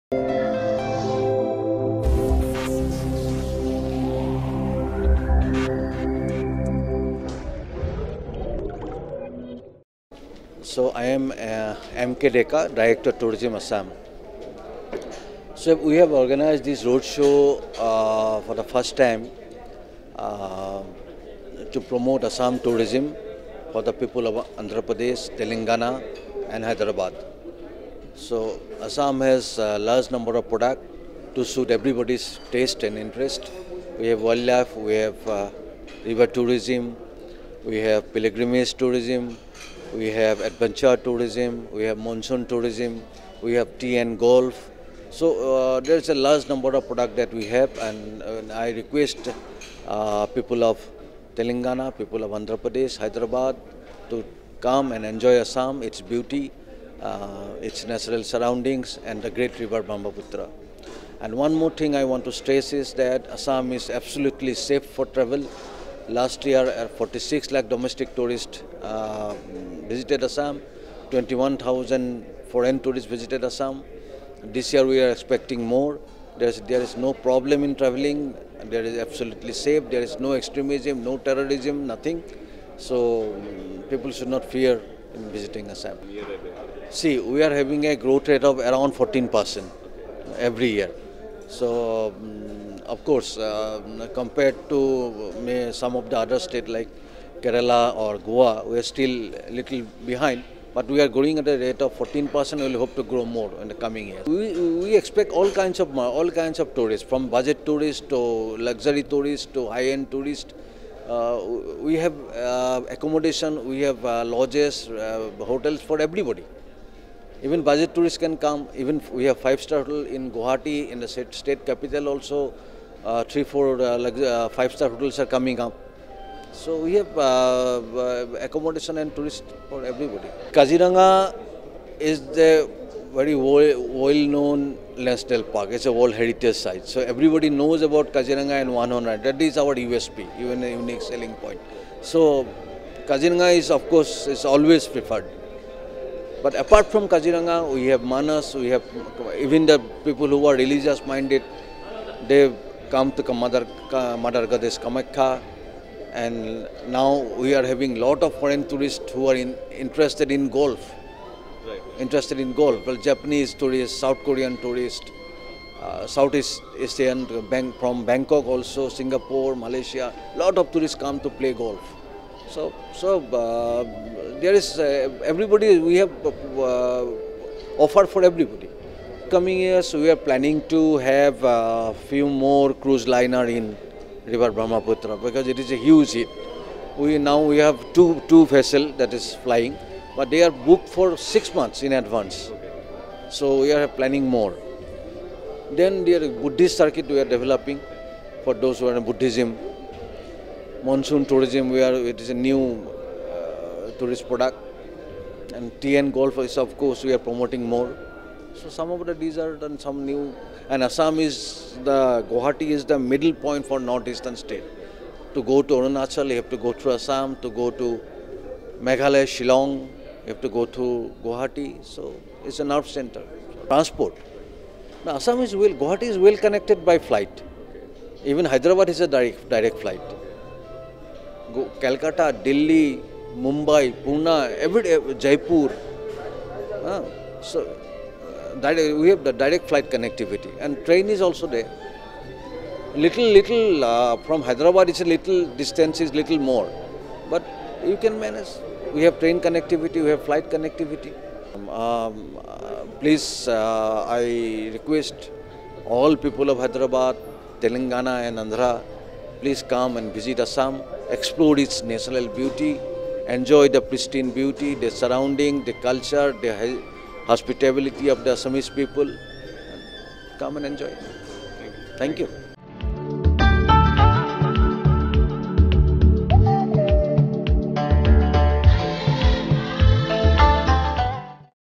Manoj Kumar Deka Assam Tourism Road Show- Hybiz.tv
Manoj Kumar Deka ACS, Director of Tourism Government of Assam Road show to Promote Destination Travel - Hybiz.tv